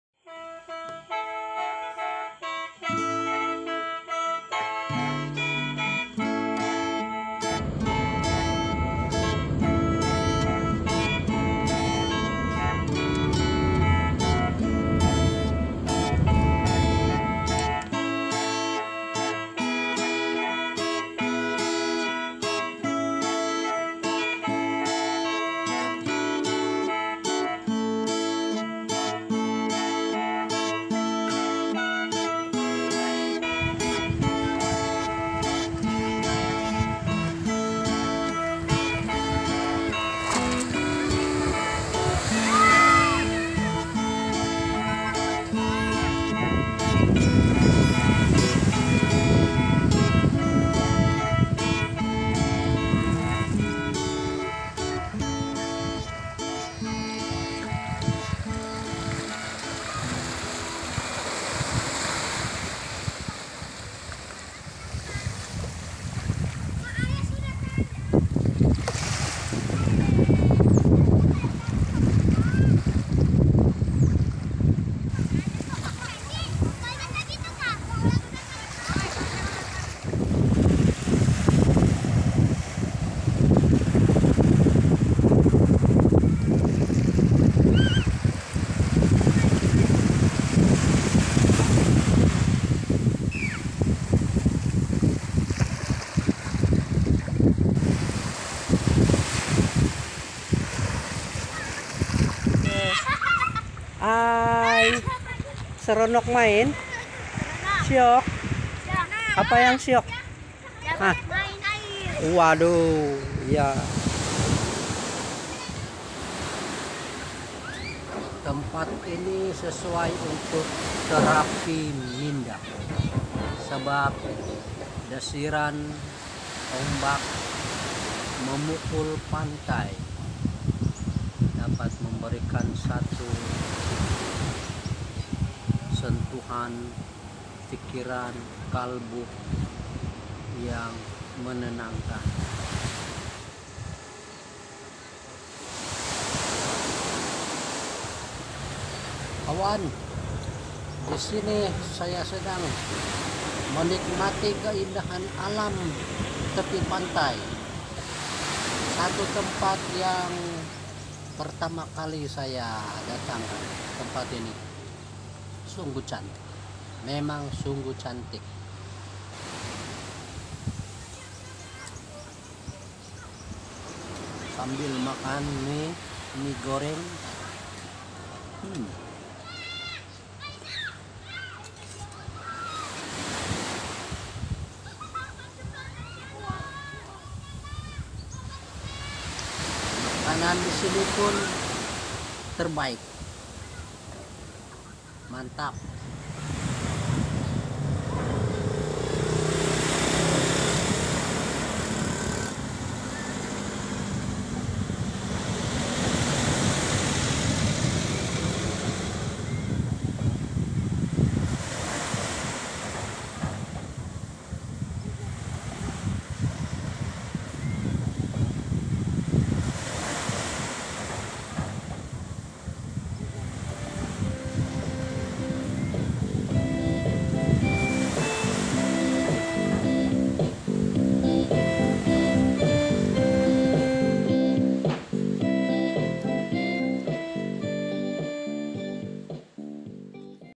TERAPI MINDA NYANYIAN OMBAK di Lok Bayu, Kuala Abai.
Perjalanan si Gaman kali ini ialah menuju destinasi sebuah tempat iaitu Lok Bayu di Kuala Abai,  Kota Belud,  Sabah.  Satu tempat yang menarik,  pemandangan yang indah, sebagai satu destinasi yang baik untuk dikunjungi.  Semoga anda terhibur dengan suasana tersebut.Desiran ombak sebagai terapi minda.

Latar muzik Jasson & Duil instrumental.